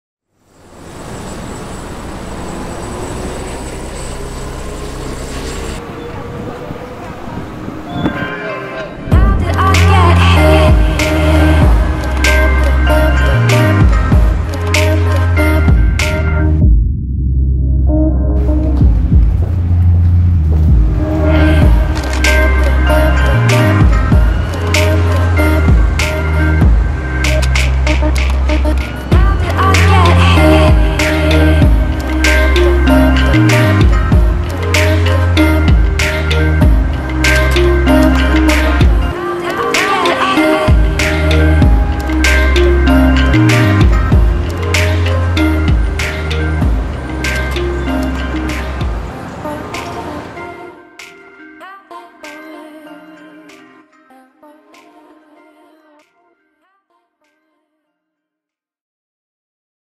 How to Travel London in 24 Hours - Canon EOS R Cinematic
Uncovering the city that never sleeps, catching some serious moody vibes from the capital. Discover an alternate perspective on London.

Shot on Canon EOS R